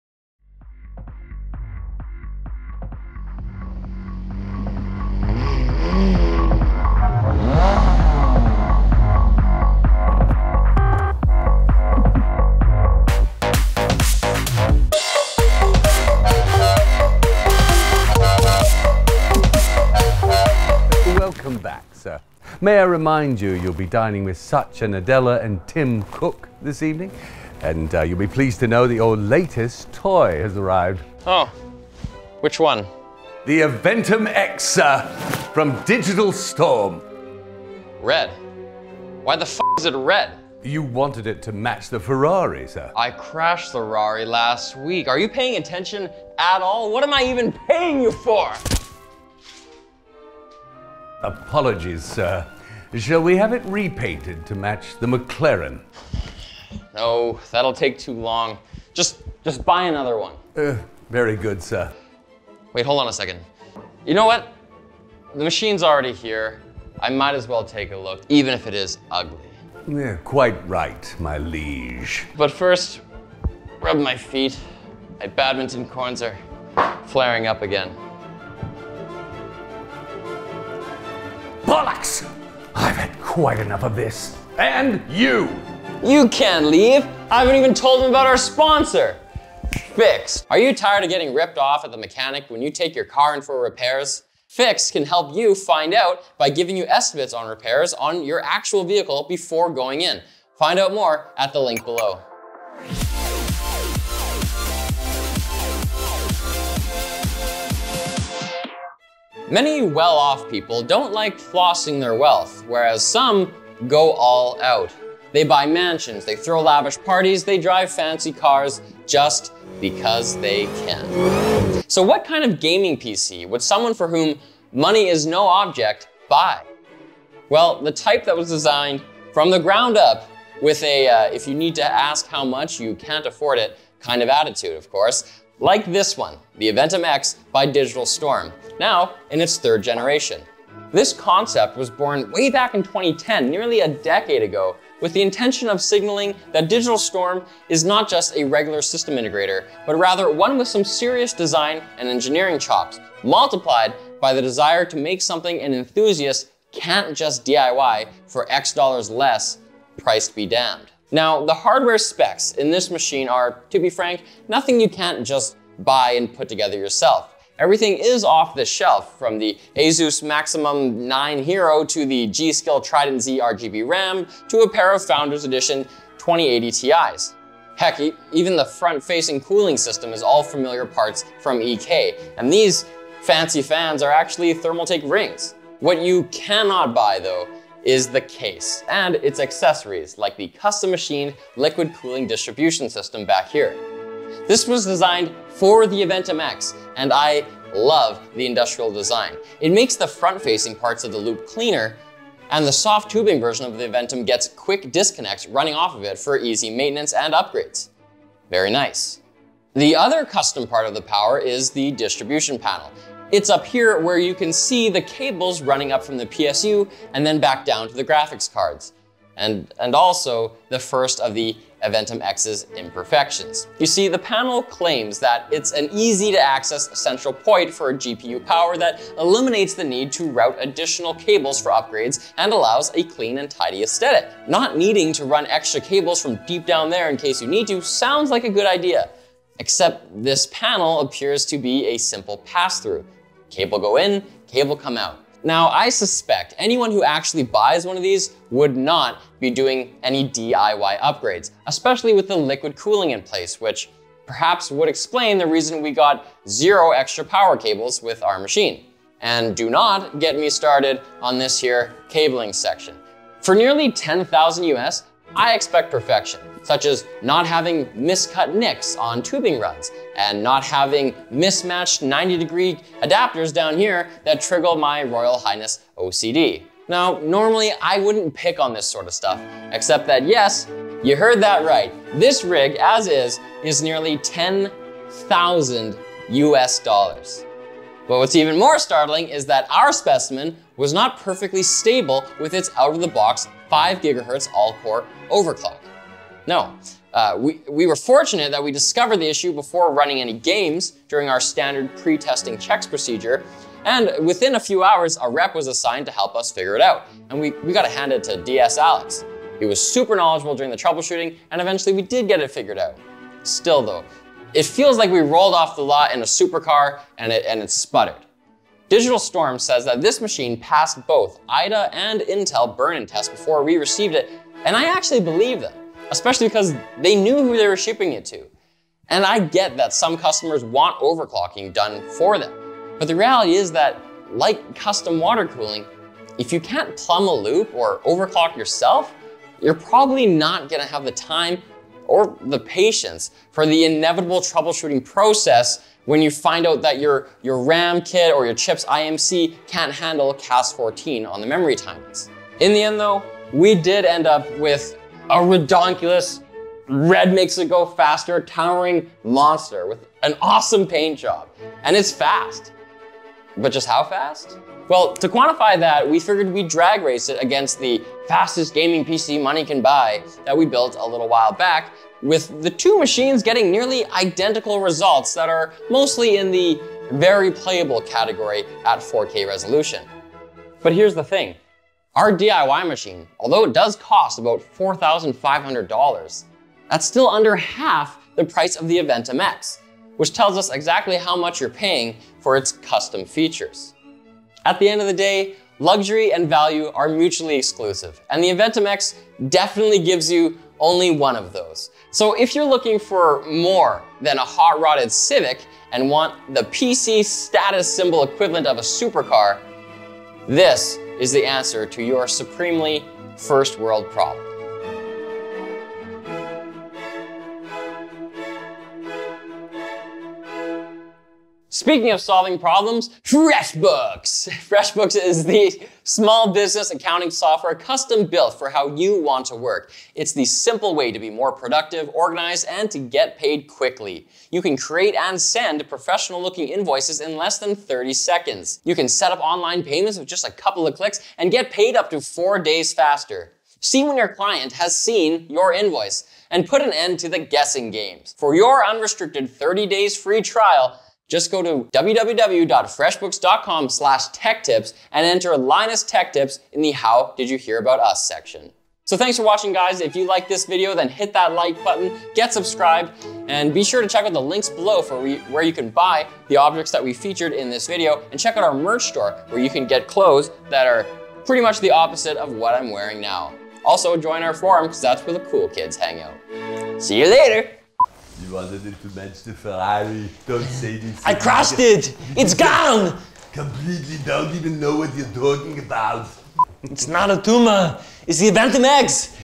The Supercar of Gaming PCs - Aventum X Review
Special thanks to the following folks for the McLaren, check them out on Instagram!:

Thanks to Richview Homes for the shoot location!

We got one of the most expensive, high-performance gaming PC’s money can buy.